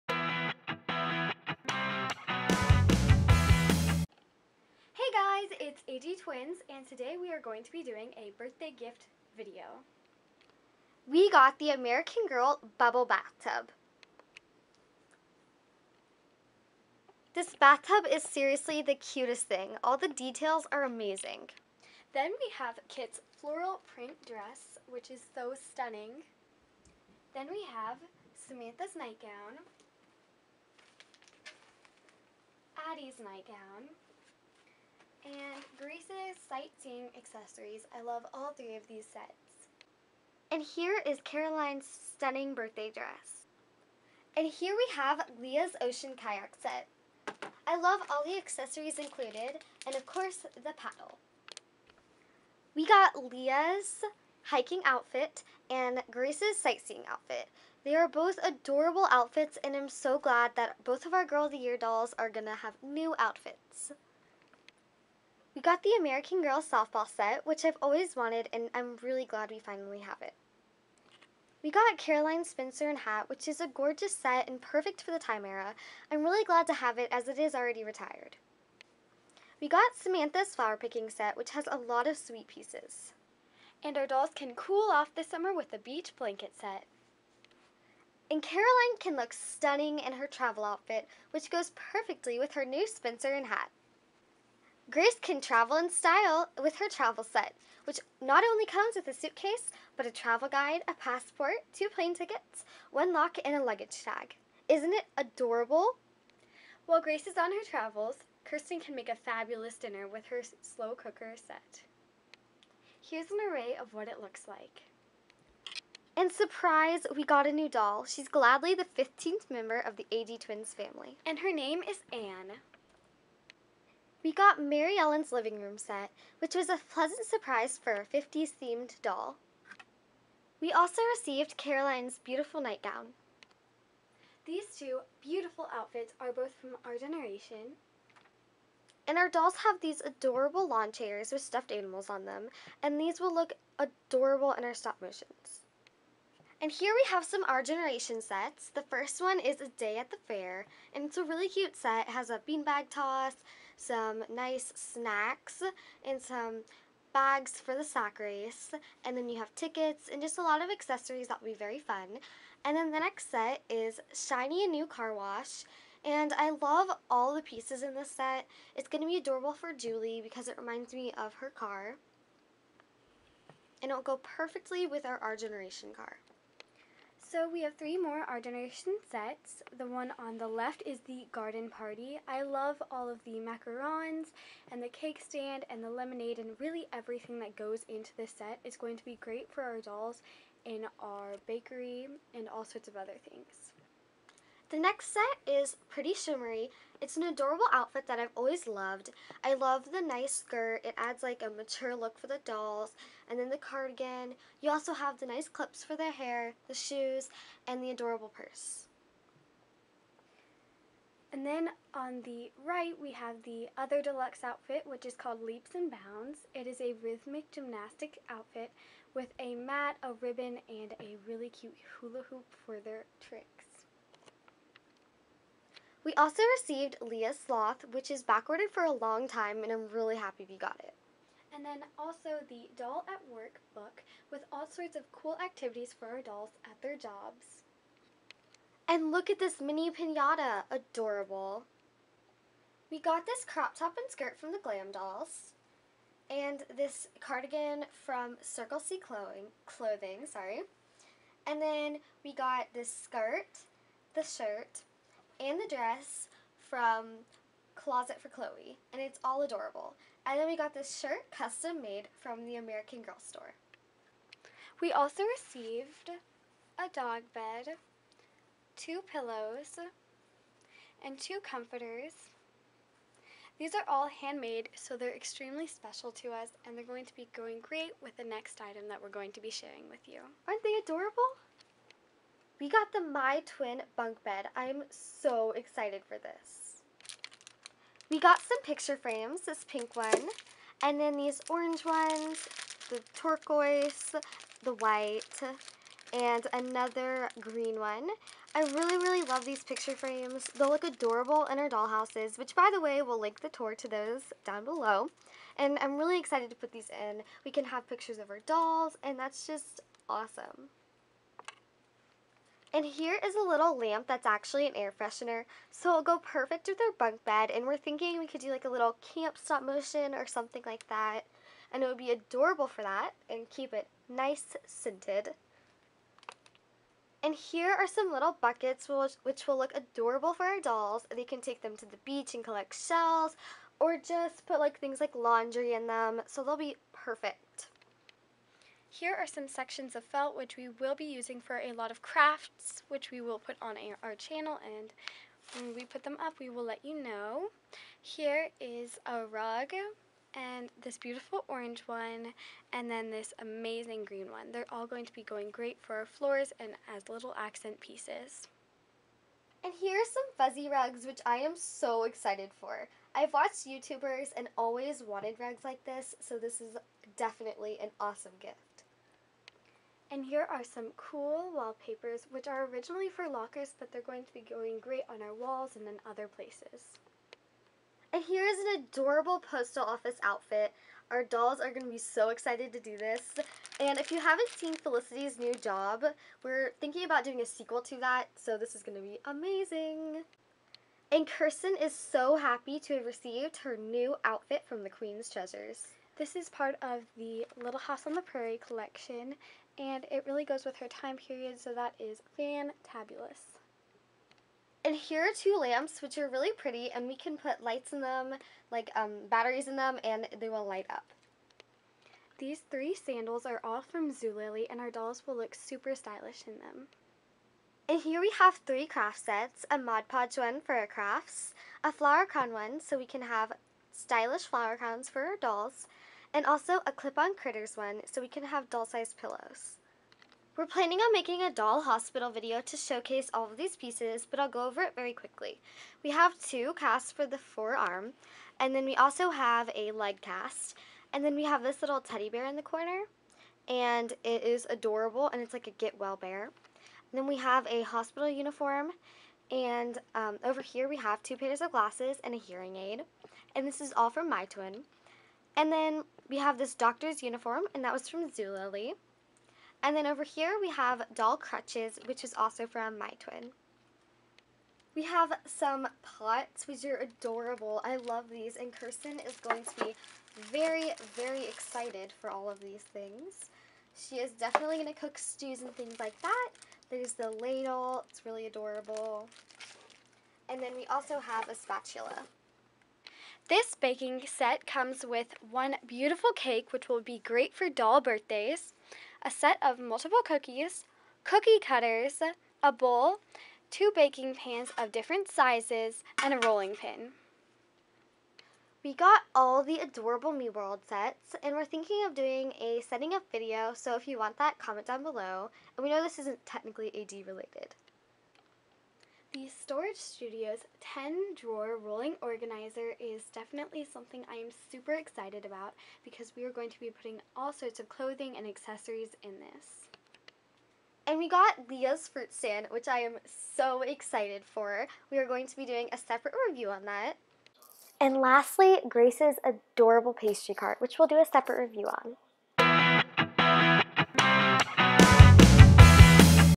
Birthday Gifts 2016!
Today we show you what we got for our bday!!! I simply love watching these videos and thought it would be fun to film!